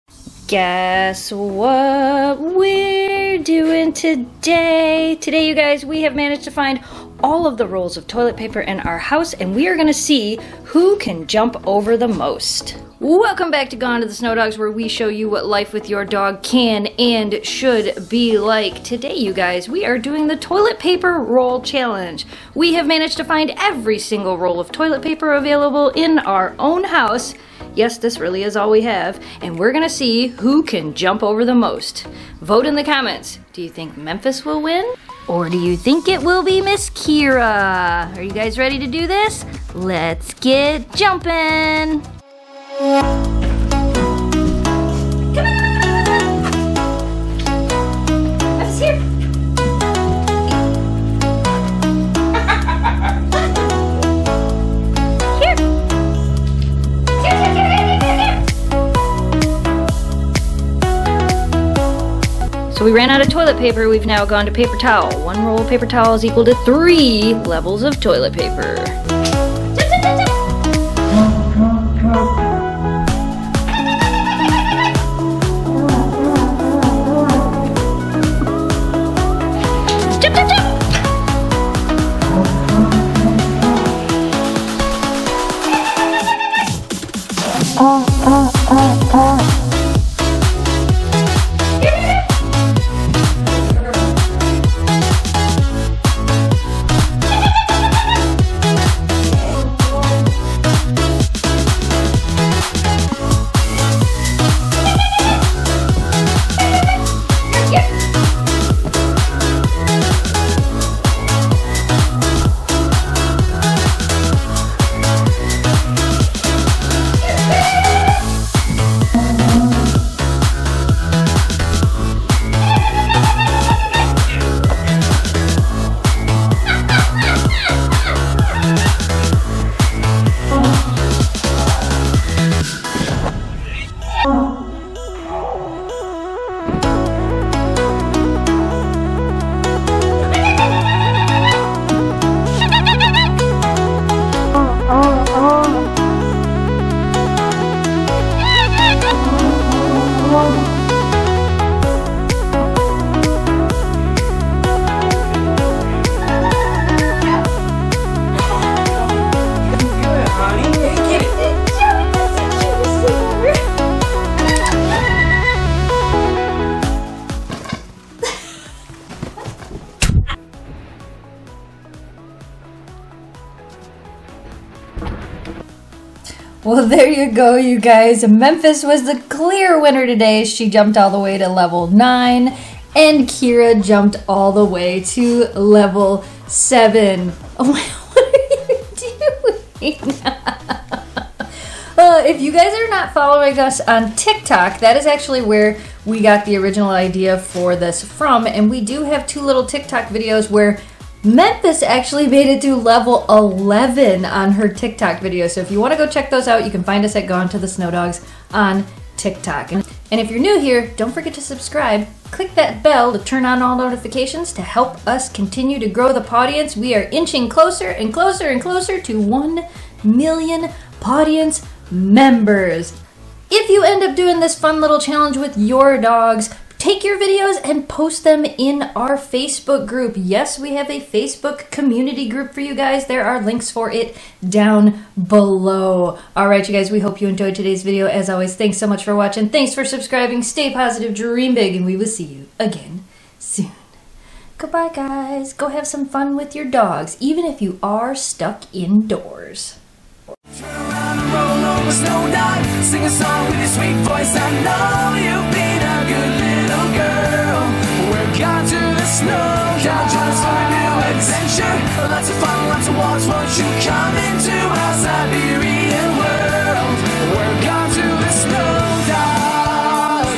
My Dog Reacts to the Toilet Paper Challenge | Huskies Vs Toilet Paper Rolls
My Dogs Reacts to the Toilet Paper Challenge! Huskies Conquer Toilet Paper Roll Challenge!  Who will win?  Stuck inside?  Need something to do?  Same!  Today the dogs are doing the Toilet Paper Roll Challenge, or as they call it on Tiktok, The Level Up Challenge!  One roll at a time, the dogs will leap over the Toilet Paper to see who can leap over the highest stack of the toilet paper rolls!  Who will win?  Let's find out!

❄Products we use

❄Send Mail to the Huskies
GonetotheSnowDogs

❄ What do We Feed Our Dogs